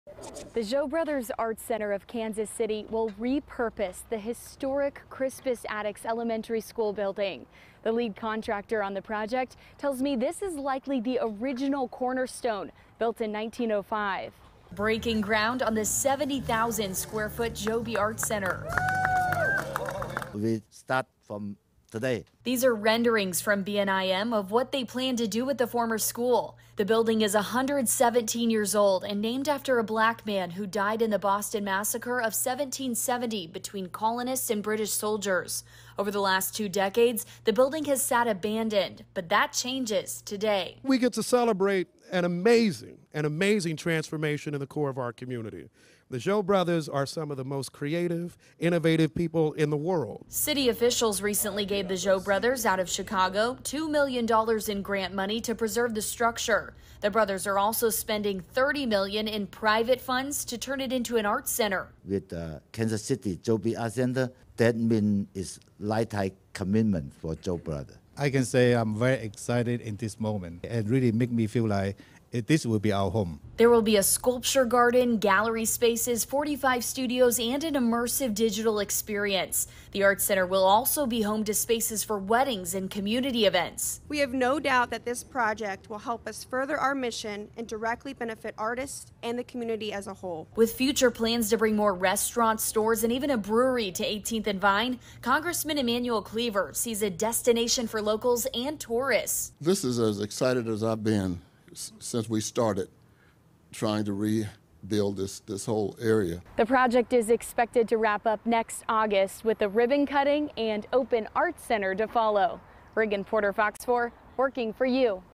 Historic building near 18th and Vine District being restored as new art center
The Zhou Brothers Art Center of Kansas City will repurpose the historic Crispus Attucks Elementary school building.